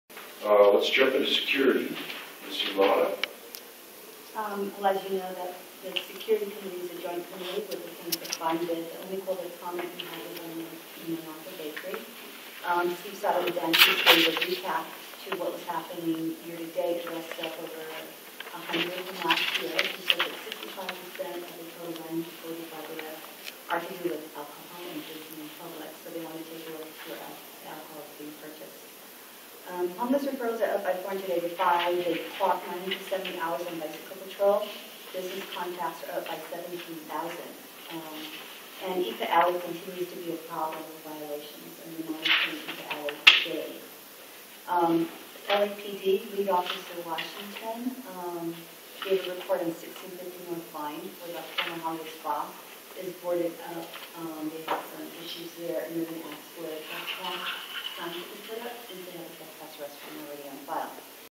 2014 10 16 First minute of M Yamada report Hollywood Entertainment BID BoD meeting
This is the first minute of Monica Yamada's report to the Hollywood Entertainment BID Board of Directors on October 16, 2014, concerning the Joint (with Sunset-Vine BID) Security Committee meeting on October 9, 2014.